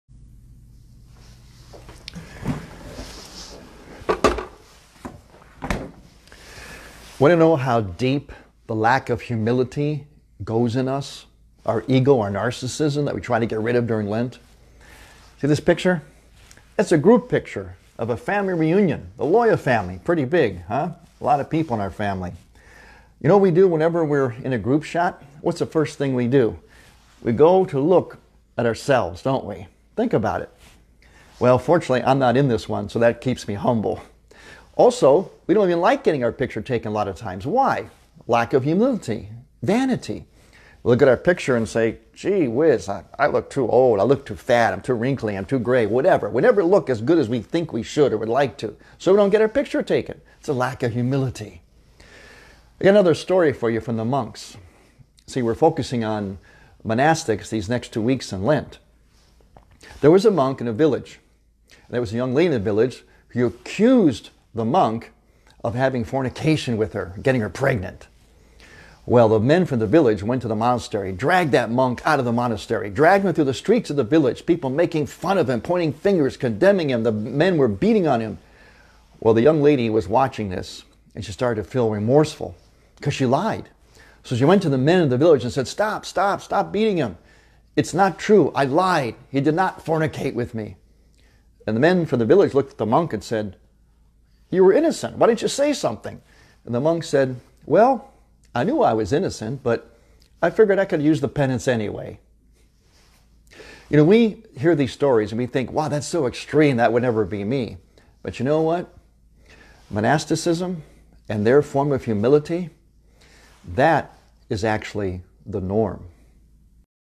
BOLD TALK 'Tabor Life" Fr.Thomas Loya - Humility and Me
Father Tom explains how monastics deal with life